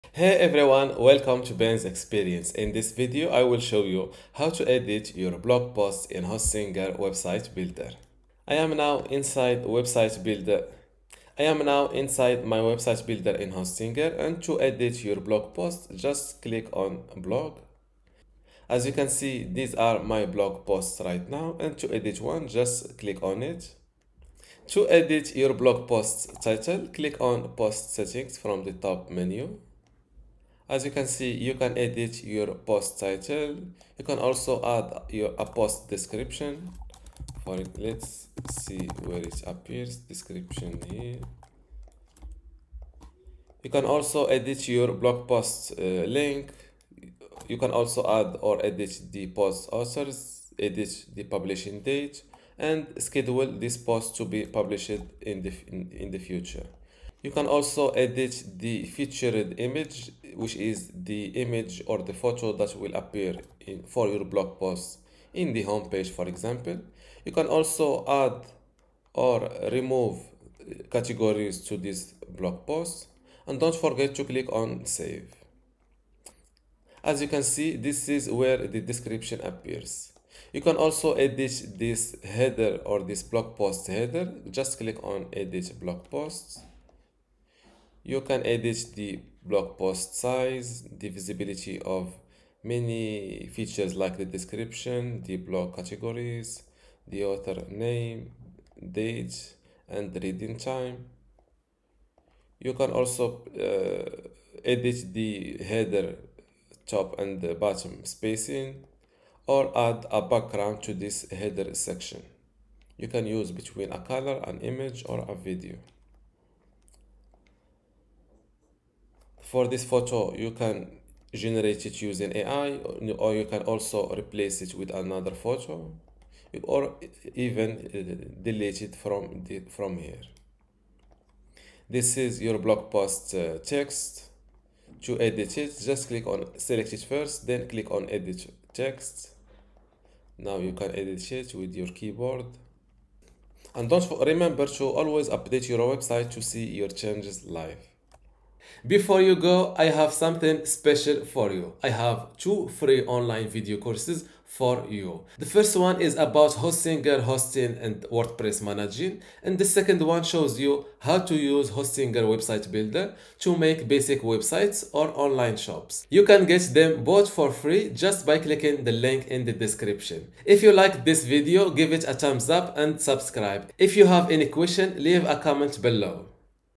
How to Edit a Blog Post in Hostinger Website Builder
In this video, I will show you how to edit your blog post in Hostinger Website Builder. Editing a blog post in Hostinger Website Builder is simple and straightforward. First, navigate to the website builder in Hostinger and click on the blog section. You will see a list of your blog posts. To edit a blog post, just click on it. You can change the blog post title by clicking on post settings from the top menu. Here, you can also add a post description, edit the blog post link, add or edit the post authors, and schedule the post for future publication.

Moreover, you can change the featured image, which appears on the homepage, and manage blog categories. Don't forget to save your changes. Additionally, you can customize the header of your blog post, adjusting elements like the size, visibility of features, and adding a background. Editing the text of your blog post is easy; just select the text and click on edit text. Make sure to update your website to see your changes live.

Editing a blog post in Hostinger Website Builder allows you to personalize your content easily and efficiently. Don't forget to check out my two free online courses on Hostinger hosting and WordPress management, and using the Hostinger website builder.